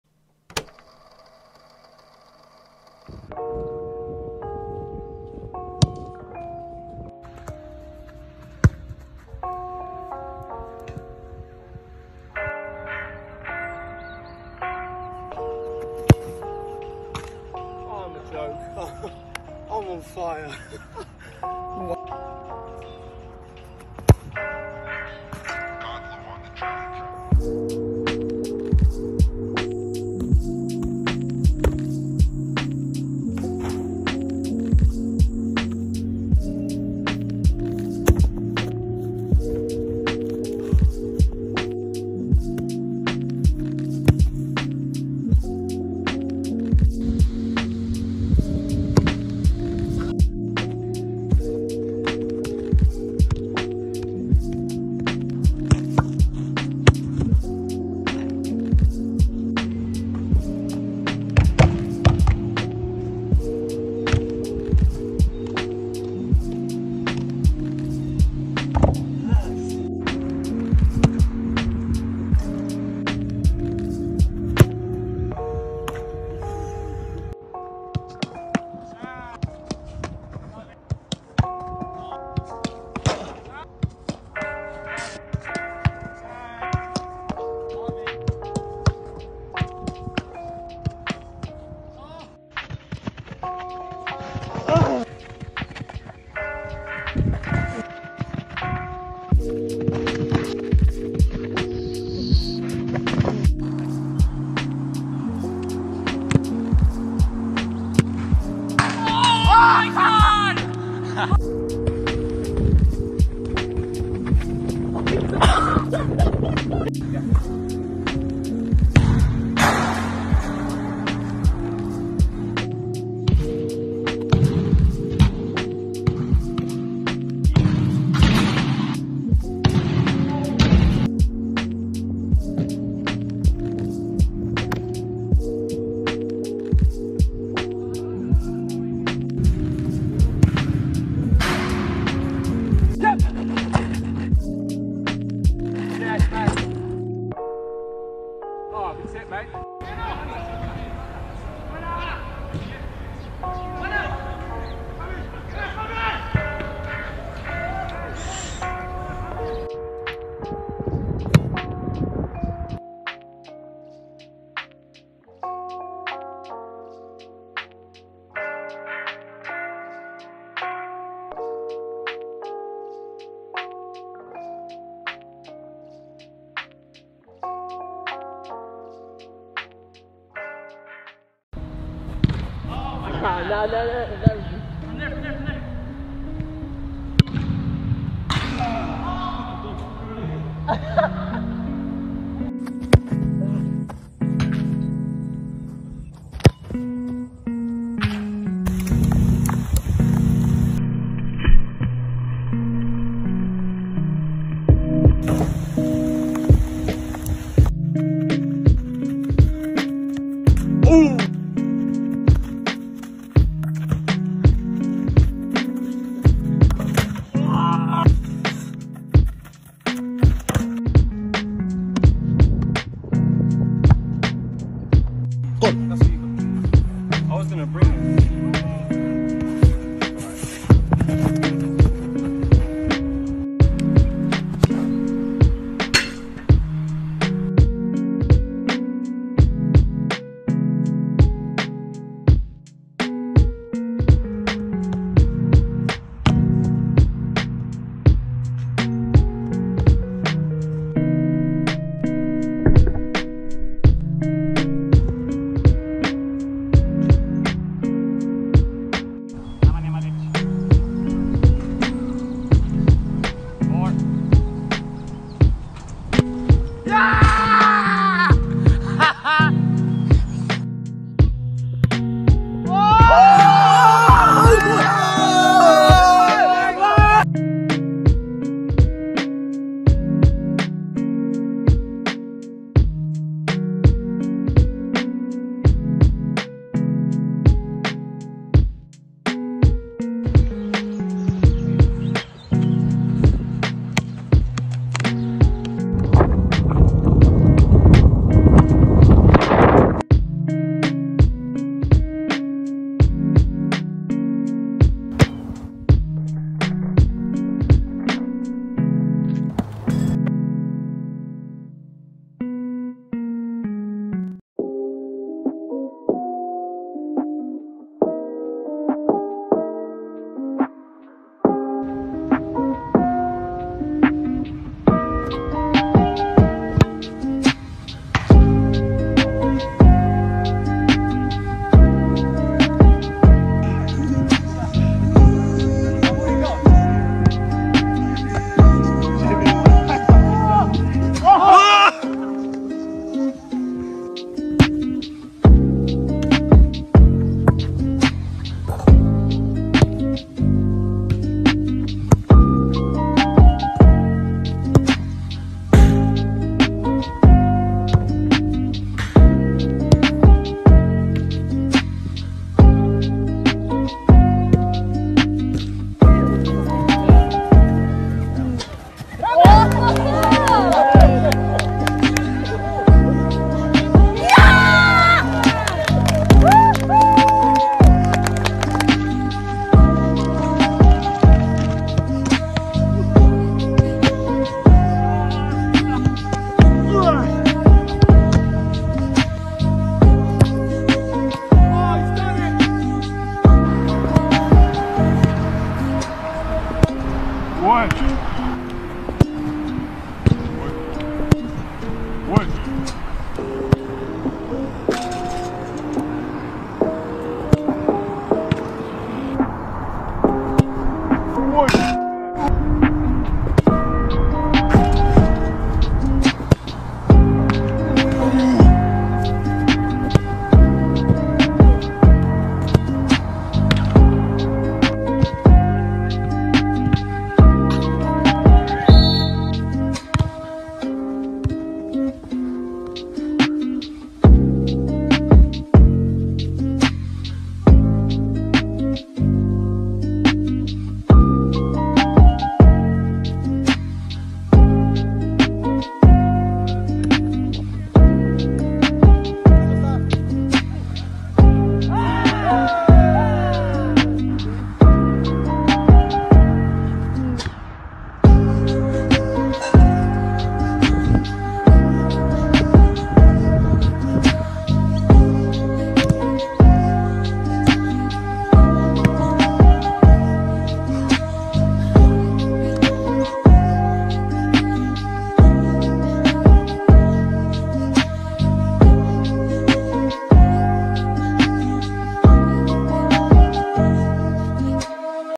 LOFI + RELAXING FOOTBALL MOMENTS - FOOTBALL SKILLS
In this video you will appreciate  a mix of LOFI Football + incredibly satisfying football shots, goalkeeper saves, passes, football skills and freestyle with a chill & relaxing Lofi beat on the background.  lofi + football moments mix, and much more.

Thank you for you understanding.